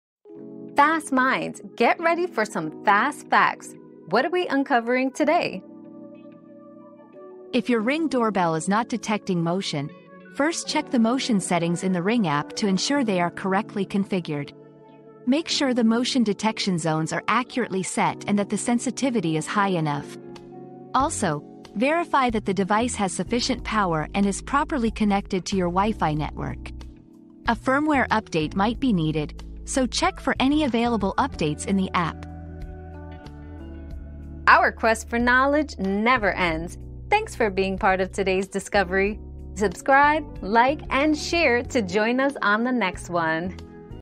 Why is my Ring Doorbell not detecting motion?
Troubleshooting Ring Doorbell Motion Detection Issues 👉 Ring Doorbell Troubleshooting 👉 Learn how to fix motion detection problems with your Ring Doorbell by adjusting settings, checking power and Wi-Fi connection, and updating firmware.

Laura S. Harris (2024, July 12.) Why is my Ring Doorbell not detecting motion?
    🌐 AskAbout.video